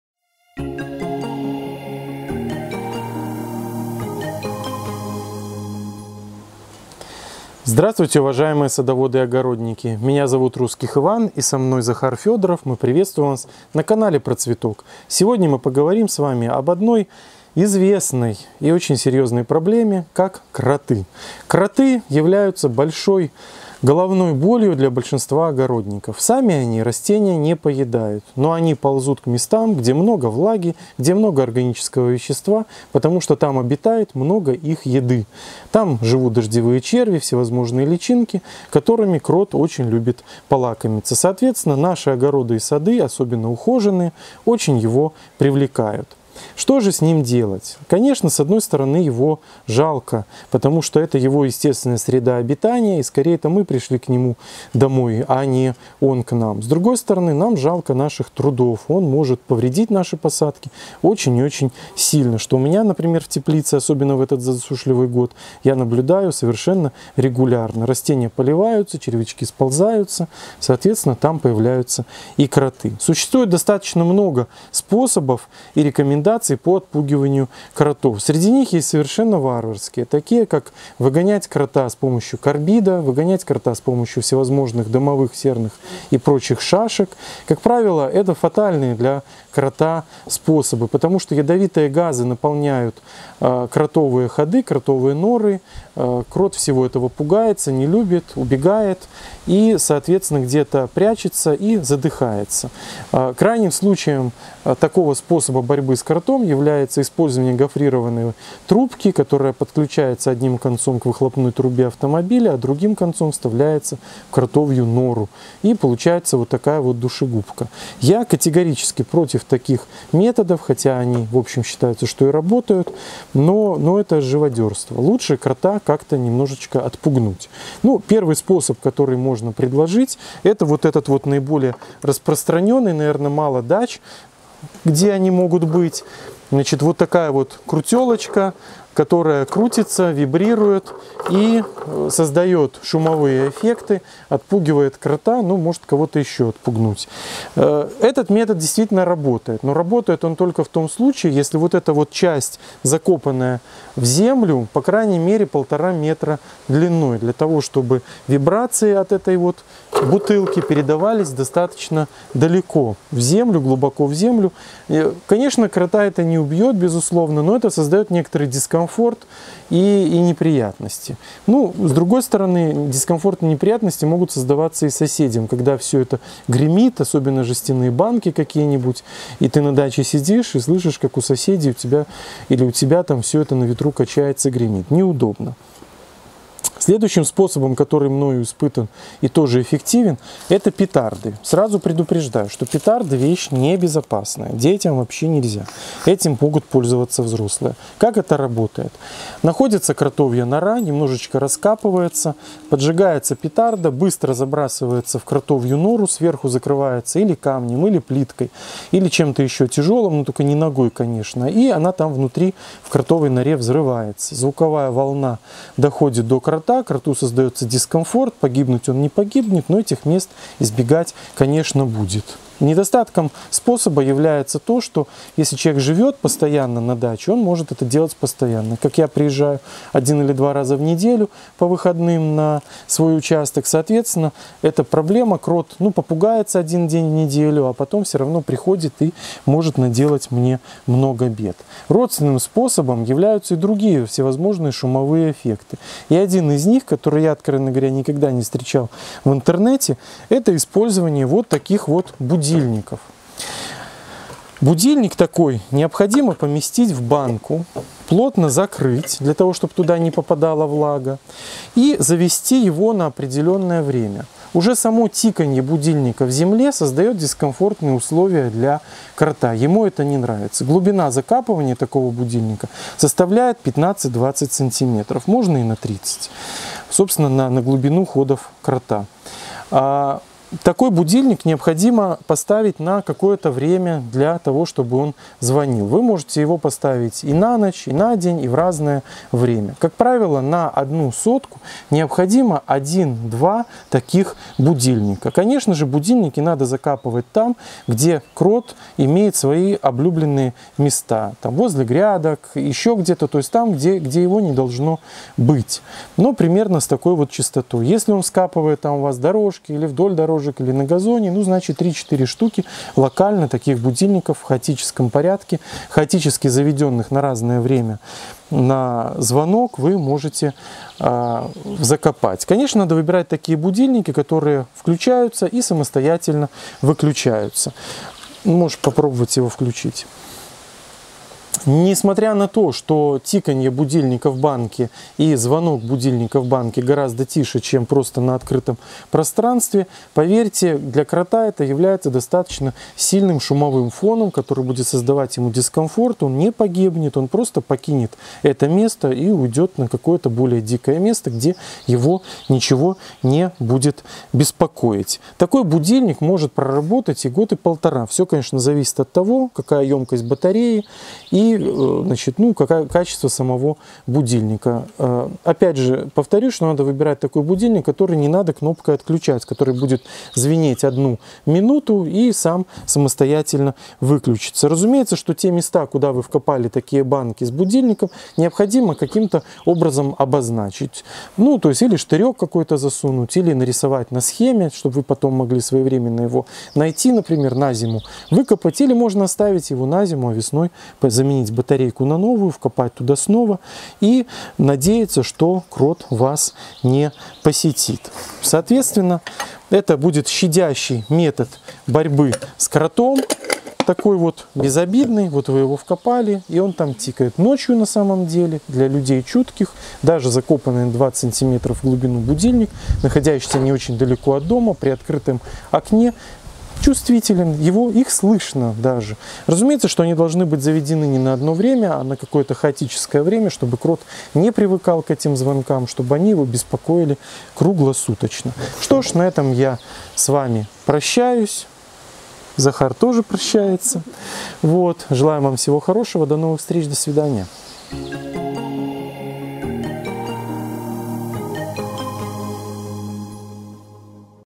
Крот в огороде. Гуманные средства борьбы. Отпугиватель кротов своими руками
Сегодня мы поговорим о том, как избавиться от крота на садовом участке. Наиболее эффективными способами побороть крота в огороде являются способы, связанные с его убийством. Мы же расскажем о гуманных способах борьбы, не предполагающих физическое уничтожение крота. Мы рассмотрим три оригинальных средства от кротов: петарды, будильник и бутылка на палке. Расскажем, как бороться с кротами эффективно, но гуманно, как сделать отпугиватель кротов своими руками Поговорим об их способах использования, преимуществах и недостатках, и вы найдете самый подходящий для себя вариант.